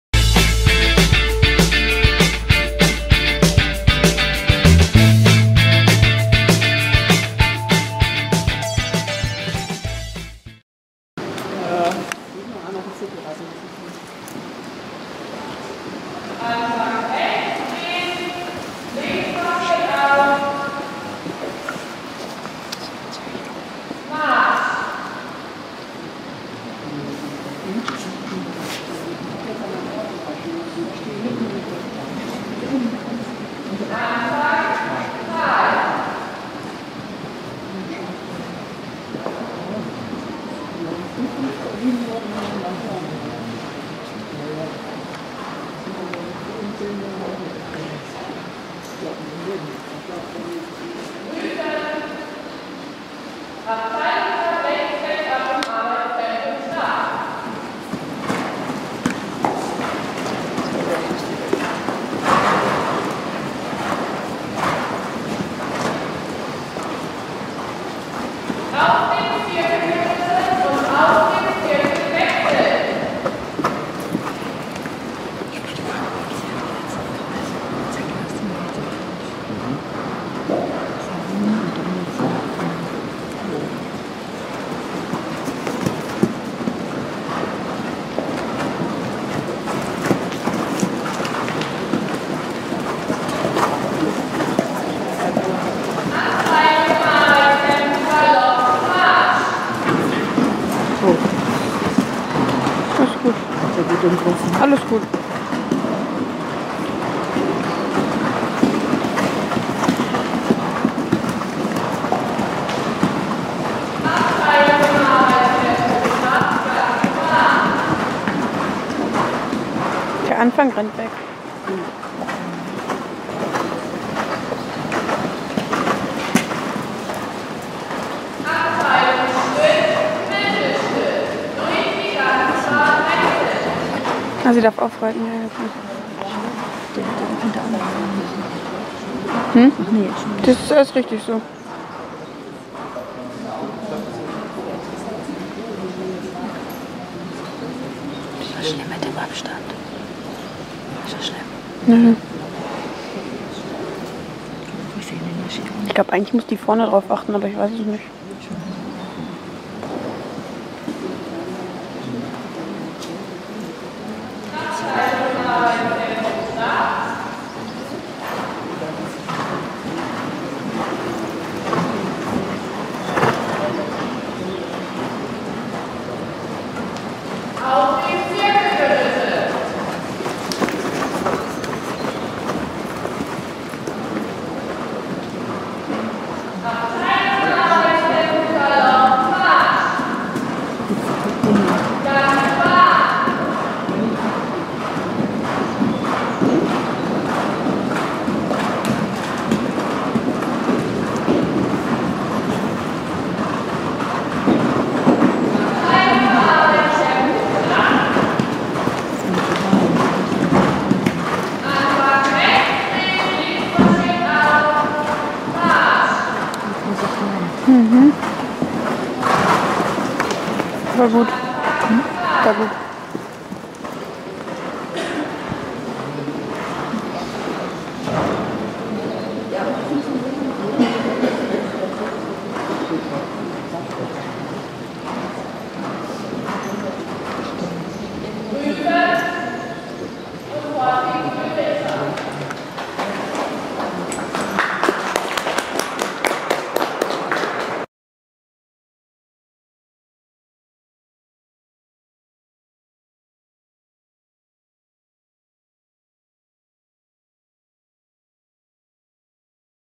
E Dressur 30.04.16 Harsewinkel mit Kossini
Hallo ihr Lieben! Dieses Wochenende lief es für uns auf jeden Fall besser als letztes Wochenende. Mit einer 6,8 konnten wir uns den 7. Platz sichern.

... ask.fm/fraeulein_pusteblume